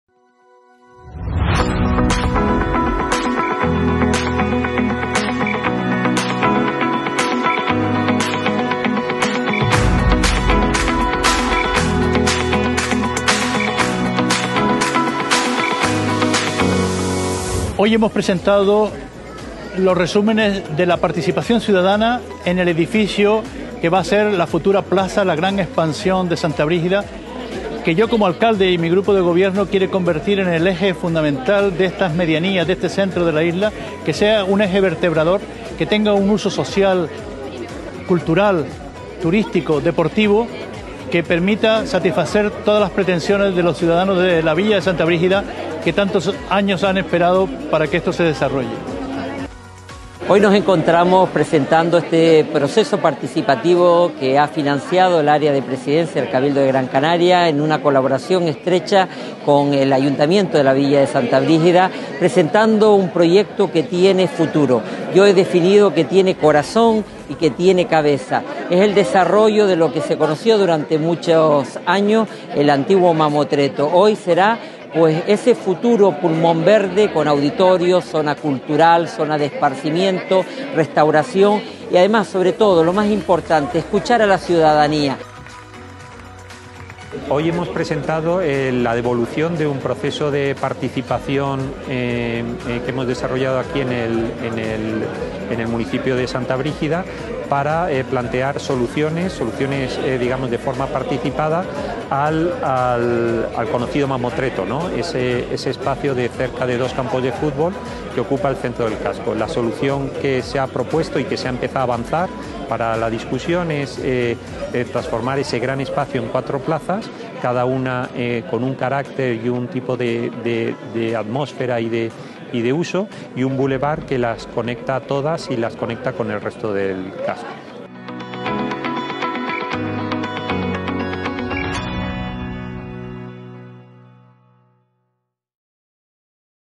Proceso participativo "Plaza de la Villa" de Santa Brígida
El proceso participativo "Plaza de la Villa", promovido y ejecutado por el Instituto 20 grados con el apoyo del Cabildo de Gran Canaria, consigue que la sociedad satauteña vea por fin la luz para el futuro del centro neurálgico de su municipio. Esta experiencia en Santa Brígida, con la colaboración imprescindible del Ayuntamiento Santa Brígida, muestra una vez más la total pertinencia del papel del Cabildo en el impulso de procesos participativos que aseguren propuestas para intervenciones públicas con viabilidad técnica y un amplio consenso social.